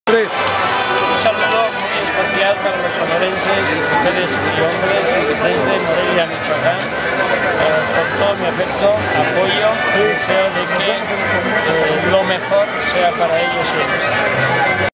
juez Baltasar Garzón con la diputada Lorenia valles
El juez español Baltasar Garzón envía un cordial saludo al estado de Sonora despues de terminar su participación el el foto sobre seguridad y desarrollo en México organizado por IDN.